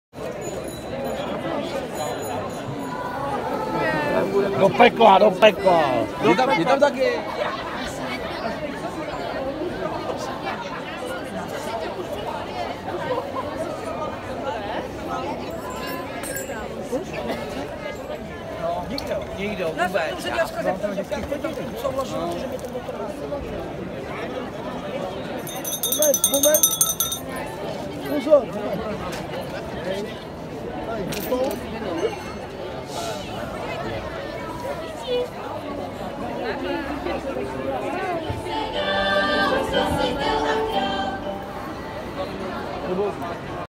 Mikuláš v Praze (St Nicholas Day in Prague)
St Nicholas Day (Mikuláš) 🎅🎄
Today you may run into St. Nicholas (Mikuláš)🎅, the Angel (anděl)👼🏻 who represents the Good, and the Devil (čert)😈 representing the Evil. All three characters walk along the streets and reward good children with sweets 🍭